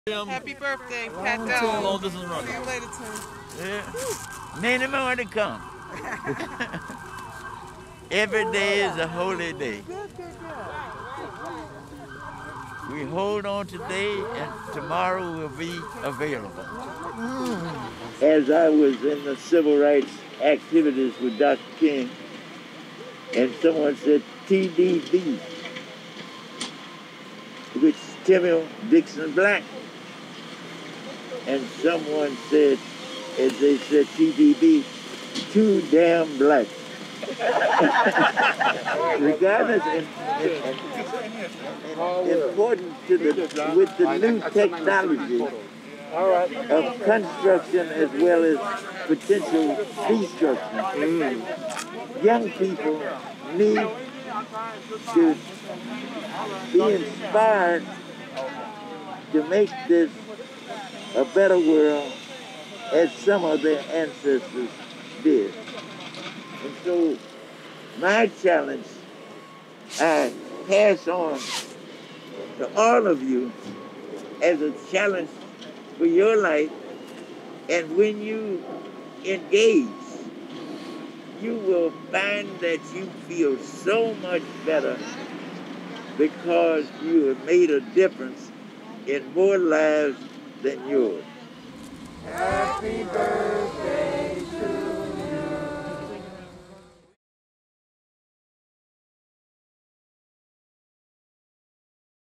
Dr. Timuel Black Celebrates 102 Birthday, Asks Young Chicagoans To Carry On Fight For Racial Justice
Dr. Timuel Black has a message to young people on his 102nd birthday in Chicago's Hyde Park neighborhood on Dec. 7, 2020.
Video by Colin Boyle/Block Club Chicago